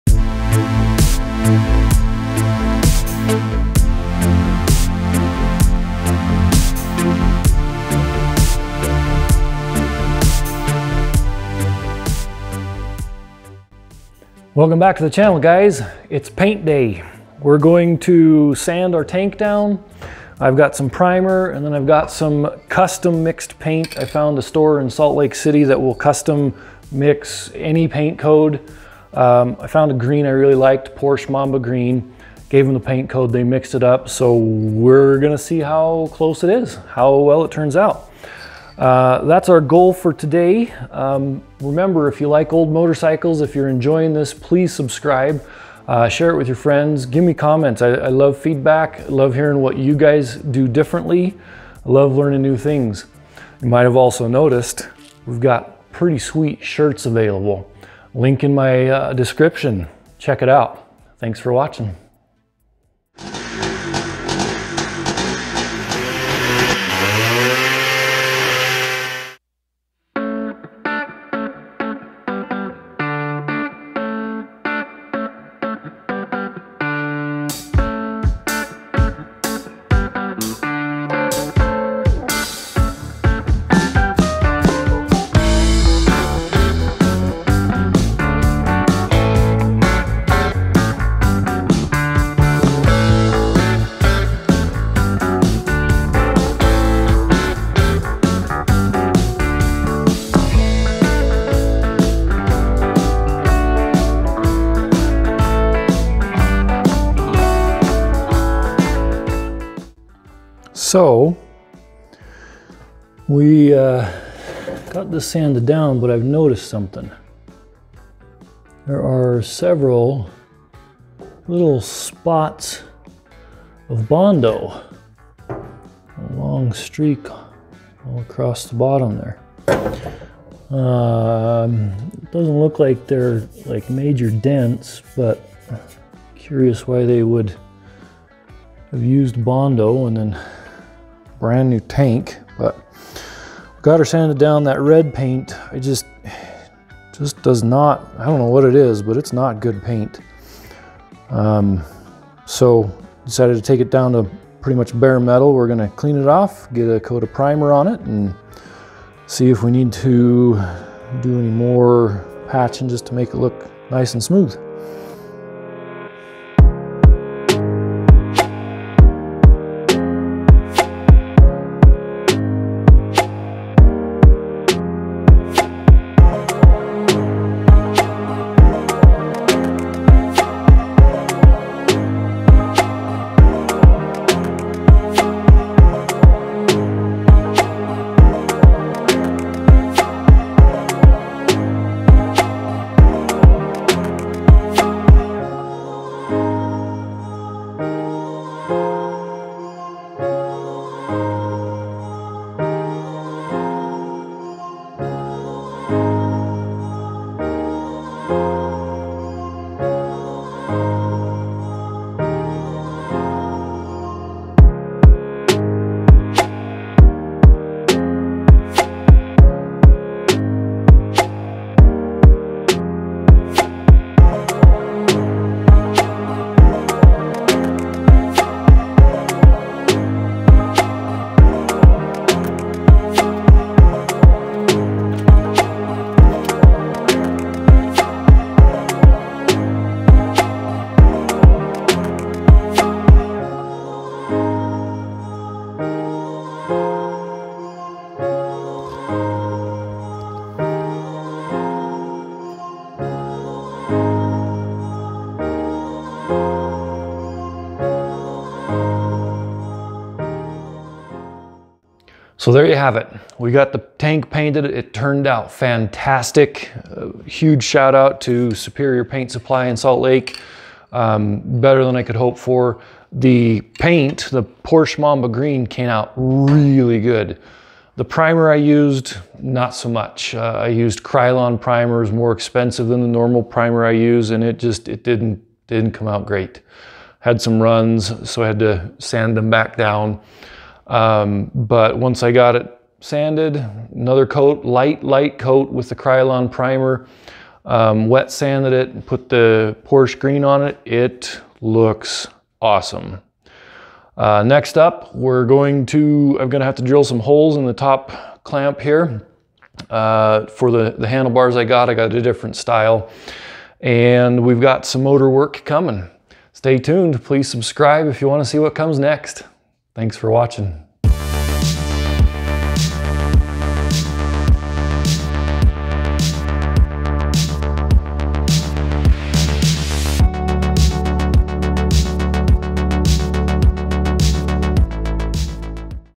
Quality paint in a spray can? ABSOLUTELY! The Mini Bike gets some Porsche Mamba Green!
I am a huge fan of rattle can paint jobs, but they can be a little limiting when it comes to custom colors.

Not anymore!!!

I found a place fairly close to me that can mix any paint code up and stuff it into a spray can!  Check them out here: Superior Paint Supply Very reasonable- this can cost about $24 with tax, but price varies depending on what all goes into the can.  I opted for a single stage because I never have had good luck with a top coat not peeling.

What color?  Porsche Mamba Green!  Turned out FANTASTIC, love it!  Paint code for this green is M6S.

Painting is fairly straightforward- Sand all the old crap paint off, smooth it out, prime it, wet sand the primer, and then do your top coat.  I ended up having to do 2 layers of primer, because the Krylon stuff I used was very runny.  Lightly sanded the runs out after it had dried, gave it another light coat of primer, then on to the wet sanding.

PATIENCE is key when working with paint!!! Let it FULLY dry before you sand, or it'll be a mess.

Links to parts and tools used in this video:

Front Fork
Tube & Tire set
Krylon Colormaxx gold paint
Chain Tensioners
Brake Shoe Set
Sprockets and Chain
Fuel tank
Fender Set
Rear Shocks
Rear Wheel Bearing and Seal
Gas Tank Rubbers
Points, condenser, and spark plug kit
Complete Gasket and Seal set
Generic bolt kit
Engine bolt kit

MORE on the Z50!